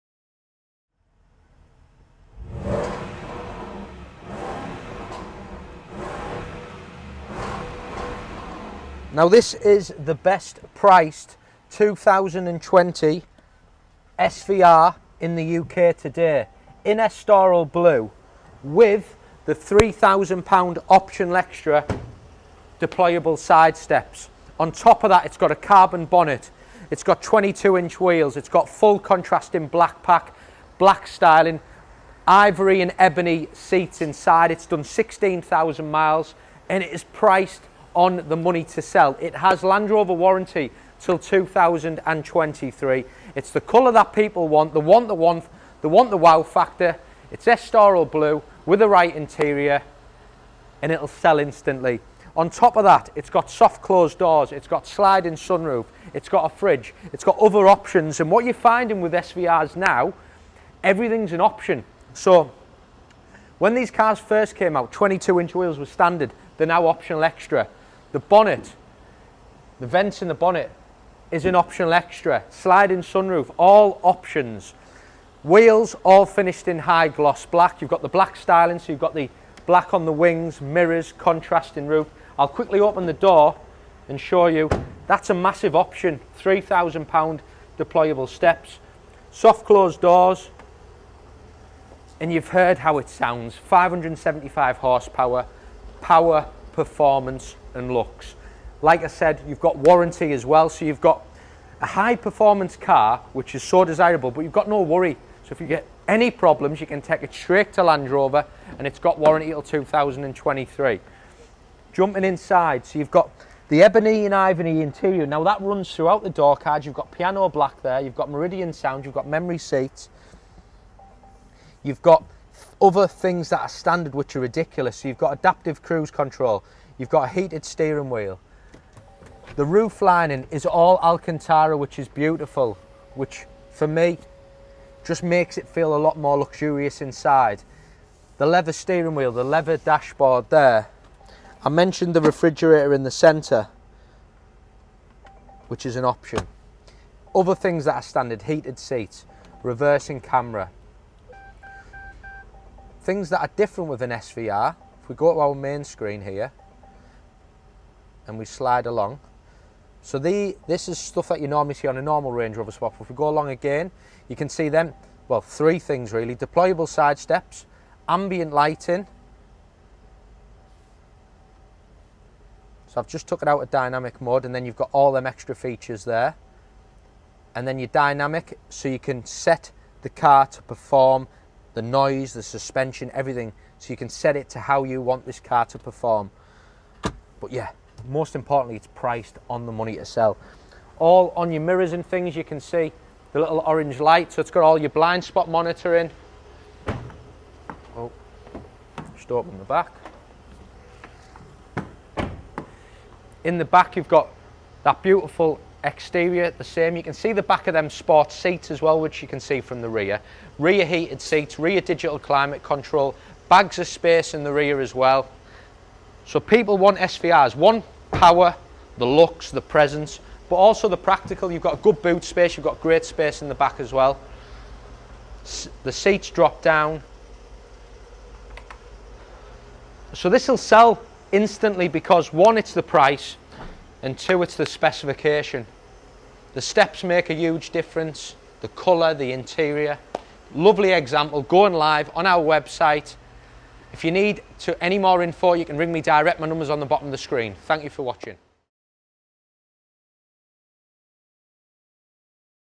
2020 Range Rover Sport 5.0 V8 SVR
An SUV that needs no introduction. From its powerful stance to its unique sound, the SVR is without a doubt the most exhilarating performance SUV available. Finished in the iconic Estoril Blue Metallic paired with a Carbon Fibre Bonnet, Black Exterior Pack Detailing, 22” Gloss Black Alloys & highly sought after Electric Deployable Side Steps

One owner from new with only 16,302 miles - Live Priced at £91,990 - Buy & Reserve Online